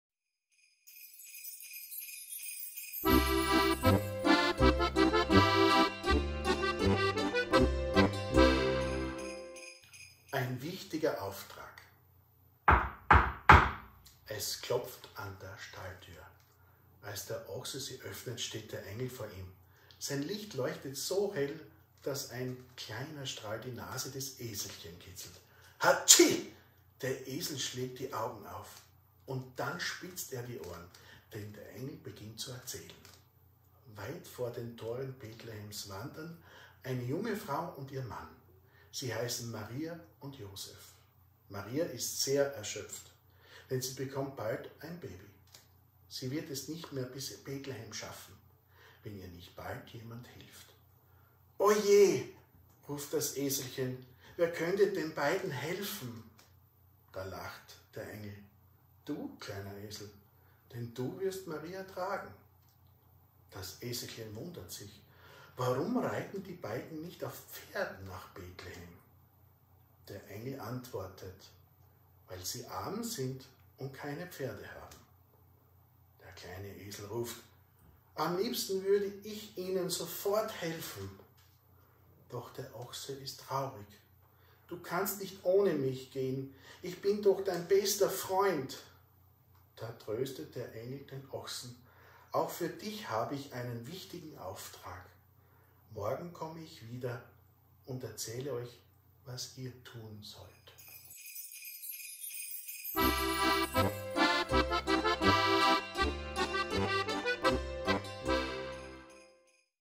#04. Teil: Ein wichtiger Auftrag.
4. Teil der Weihnachtsgeschichte
Vorgetragen von Dir. Wilhelm Berghold